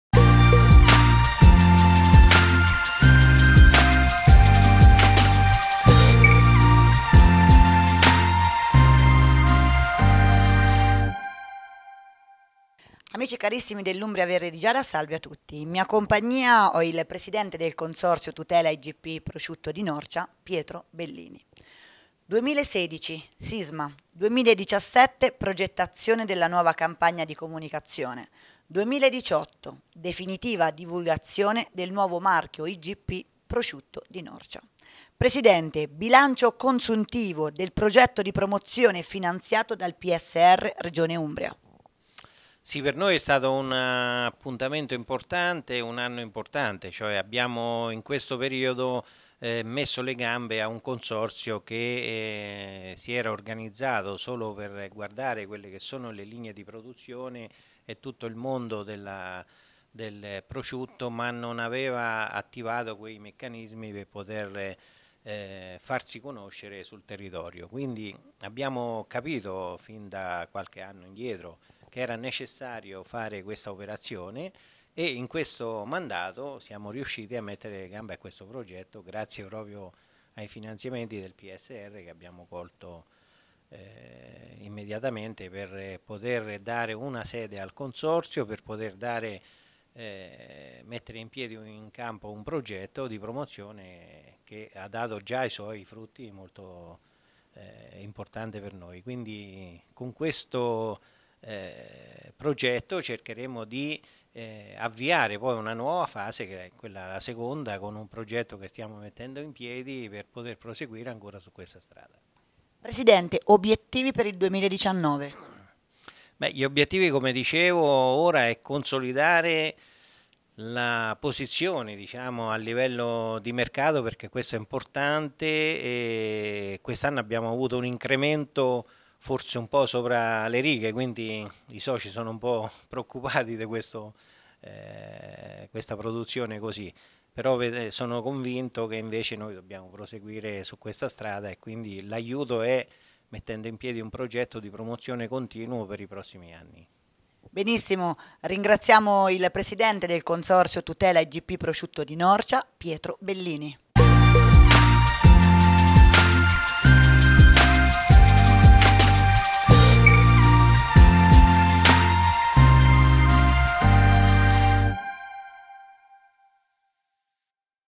CONSORZIO TUTELA E VALORIZZAZIONE PROSCIUTTO IGP NORCIA
Consorzio Tutela e Valorizzazione Prosciutto IGP Norcia in collaborazione con l' Umbria Verde di Giada.

Il Ringraziamento va al Presidente del Consorzio Pietro Bellini

2016 SISMA

2017 PROGETTAZIONE DELLA NUOVA CAMPAGNA DI COMUNICAZIONE

2018 DEFINITIVA DIVULGAZIONE DEL NUOVO MARCHIO IGP DEL PROSCIUTTO DI NORCIA

- Bilancio Consuntivo del Progetto di Promozione Finanziato dal
  PSR Regione Umbria.

- Obiettvi 2019

Info:
Consorzio di Tutela del
Prosciutto di Norcia IGP
Via Solferino, 26
Norcia PG 06046
Italia